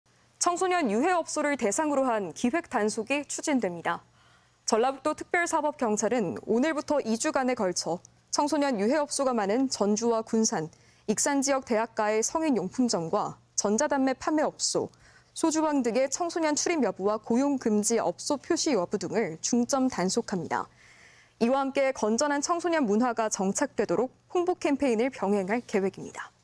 전라북도 특사경, 6월 2일까지 청소년 유해업소 단속 | 전주MBC 230522 방송
청소년 유해 업소를 대상으로 한 기획단속이 추진됩니다.

전라북도 특별사법경찰은 오늘(22일)부터 2주간에 걸쳐 청소년 유해업소가 많은 전주와 군산, 익산지역 대학가의 성인용품점과 전자담배 판매업소, 소주방 등의 청소년 출입여부와 고용금지 업소 표시 여부 등을 중점 단속합니다.

이와 함께 건전한 청소년 문화가 정착되도록 홍보 캠페인을 병행할 계획입니다.